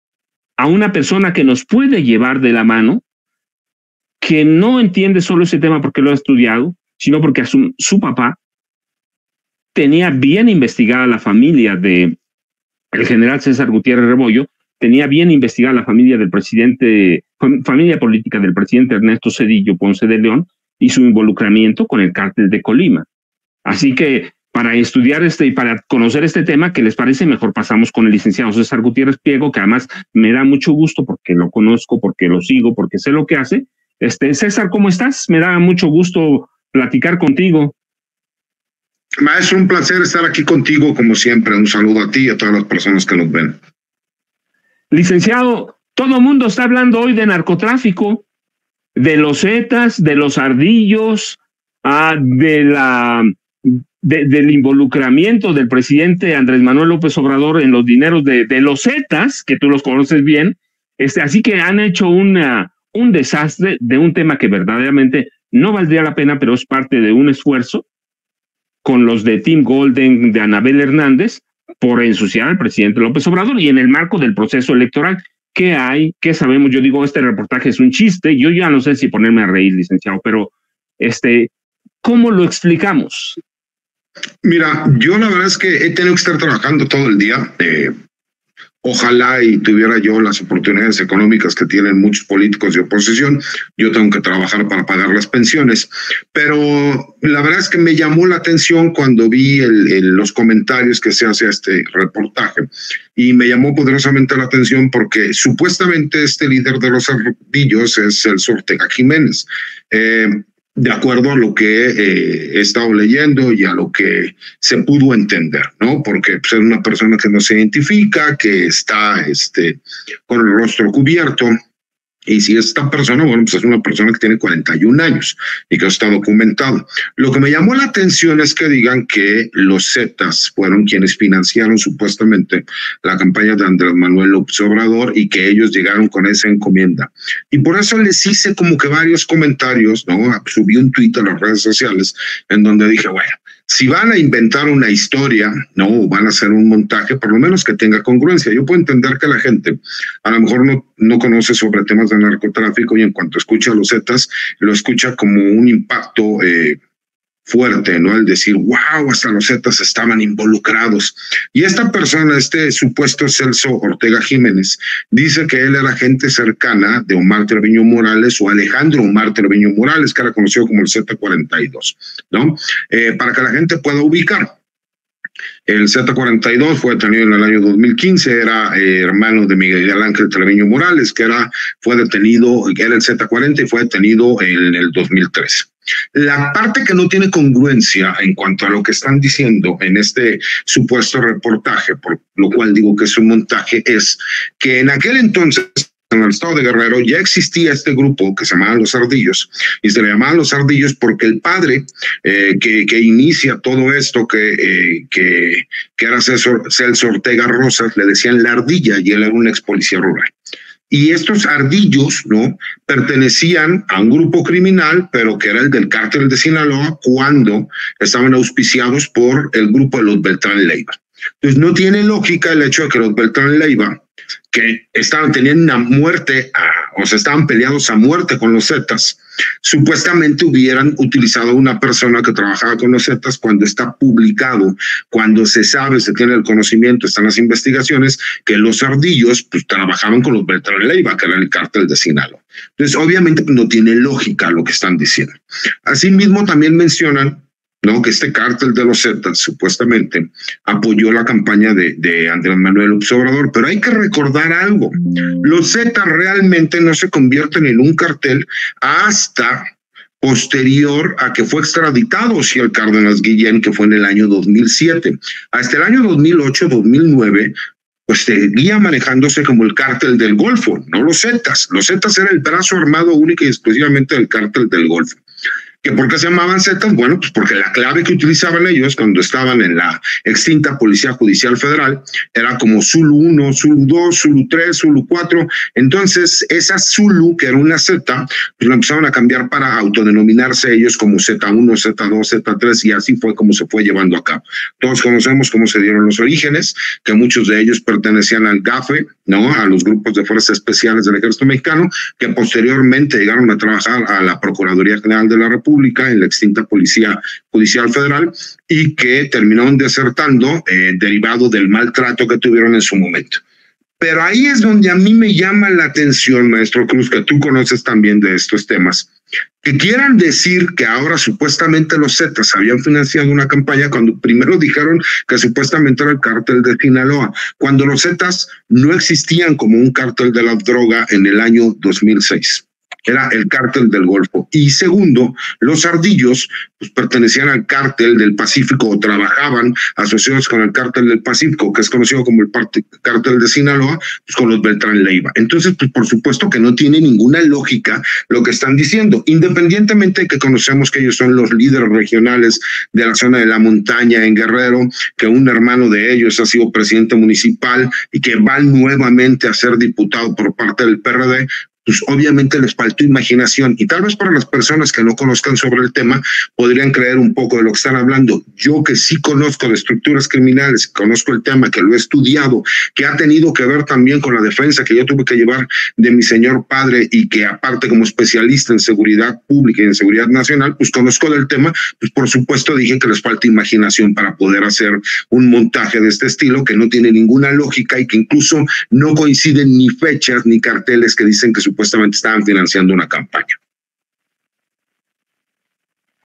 LoMejorDeSinCensura | CÉSAR G. TIRA EL REPORTAJE DE LORET, ¡NO TIENE NI CONGRUENCIA!
El abogado César Gutiérrez Priego se refirió al reportaje que dio a conocer Loret de Mola en su canal Latinus y señala que la historia no tiene ni congruencia, porque no coinciden fechas ni c4rt3l3s que supuestamente financiaron la campaña del Presidente Andrés Manuel López Obrador ¡Les falta imaginación!

César Gutiérrez Priego: Abogado, especialista en Derecho Penal, Derecho Militar y Seguridad Nacional